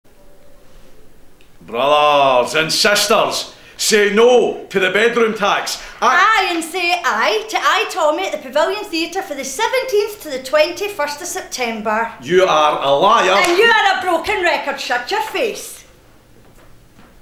Tommy Is interrupted
Rosie Kane interrupts a Tommy rant